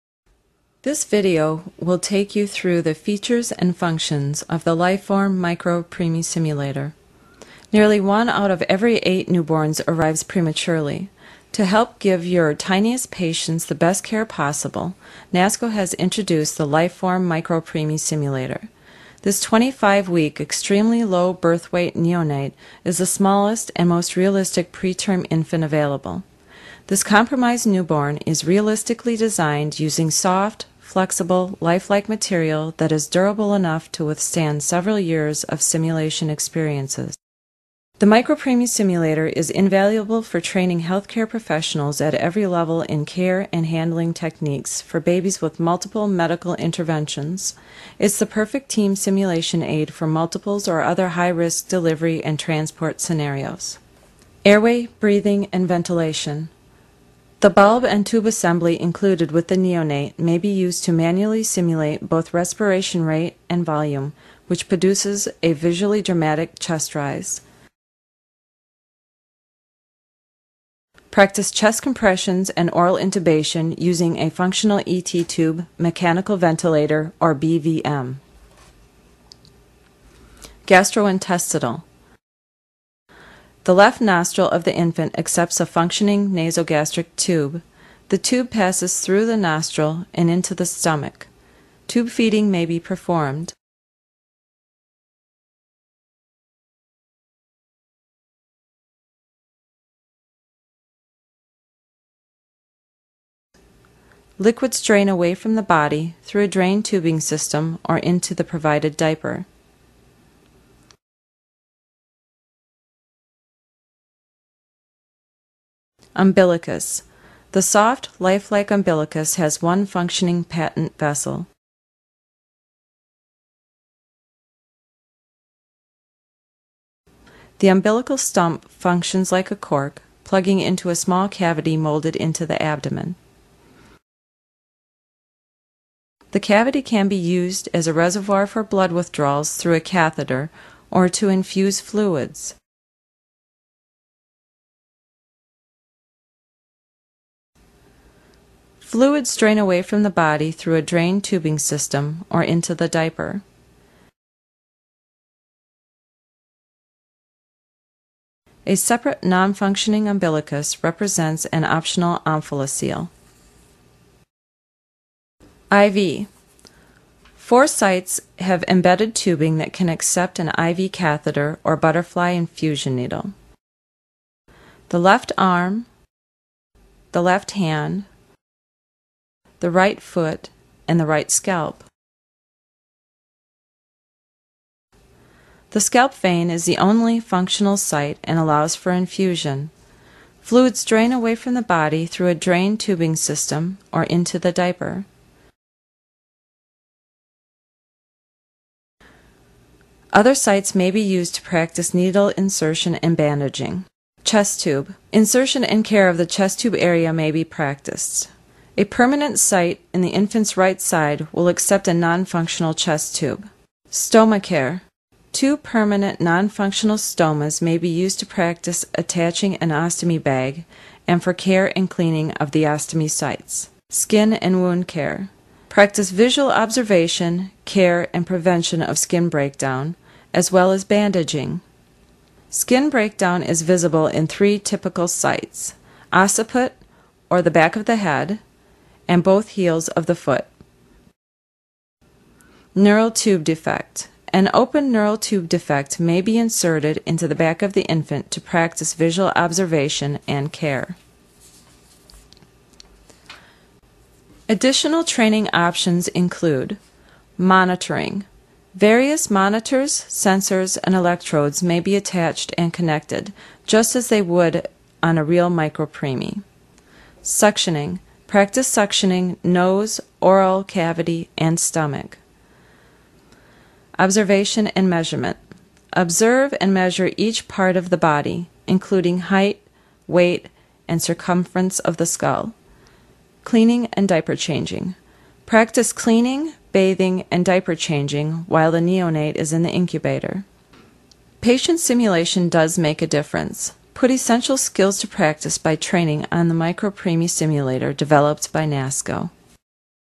preemie_baby_Nasco.wmv
Fantastic tutorial video on the Nasco Life/form® Micro-Preemie Simulator.

Nearly one out of every eight newborns arrive prematurely. To help you give your tiniest patients the best care possible, Nasco is proud to introduce the Life/form® Micro-Preemie. This 25-week ELBW (Extremely Low Birth Weight) neonate is the smallest and most realistic pre-term infant available. Invaluable for training nurses and parents in care and handling techniques for babies that have had multiple medical interventions; the perfect team training aid for multiples or other high-risk delivery and transport scenarios. The baby can be ventilated and will accept a functional ET tube, NG tube, umbilical catheter, and IV line, as well as a non-functional chest tube and stomas. Detachable neural tube defect and omphalocele. Includes diaper, hat, lubricant, 3 cc syringe, 25 G. butterfly needle and blood powder. Hat color may vary. NG tube size 5 FR. ET tube size 5 mm ID. Simulator dimensions: 12" long, 4" deep, and 18-1/2" across from hand to hand. Three year warranty.